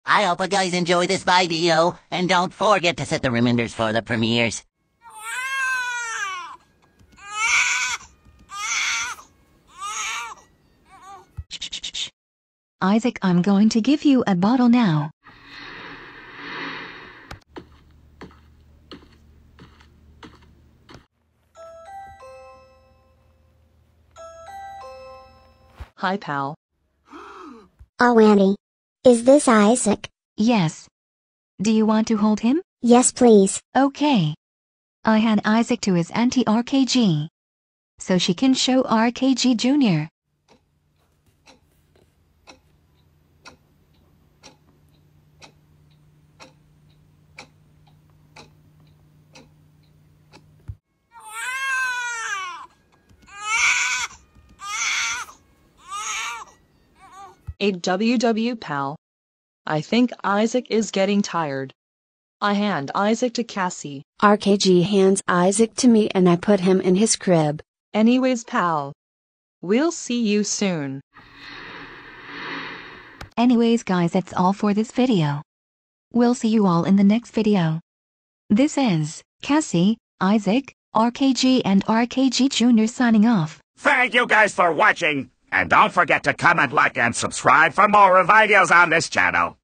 and RKG jr meet Isaac/they hold him.
In today’s video it’s @RKGTheDreamyLucarioFanGirl2025, RKG jr, Isaac and cassie

Note: from the part where I say: @RKGTheDreamyLucarioFanGirl2025  hands Isaac to me and I put him in his crib. I forgot to switch from the Saill voice to Julie. So my voice is Saill in that sentence. So just a heads up.